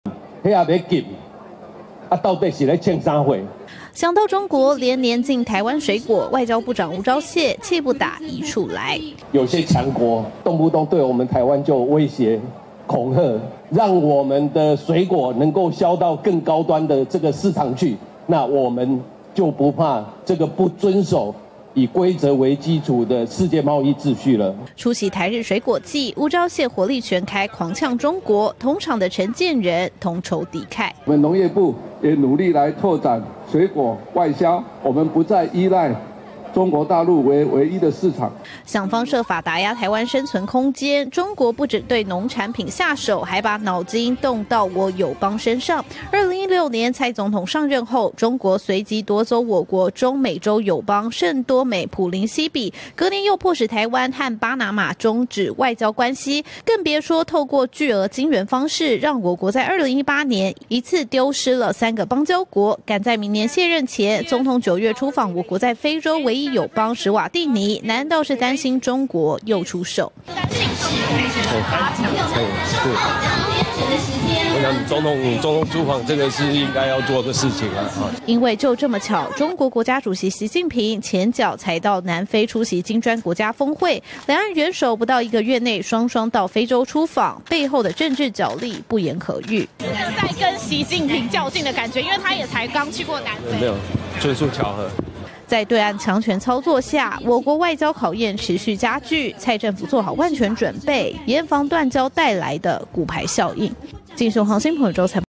總統出訪非洲與習近平較勁？吳釗燮駁：純屬巧合｜鏡新聞
蔡英文總統將在9月5日至8日出訪非洲友邦史瓦帝尼，由於中國國家主席習近平剛訪問南非，出席金磚五國峰會，外界解讀有較勁意味。對此，今天（26日）天下午出席台日水果祭的外交部長吳釗燮解釋，只是純屬巧合，卻在致詞時火力全開，批評中國，動不動禁台灣水果恐嚇。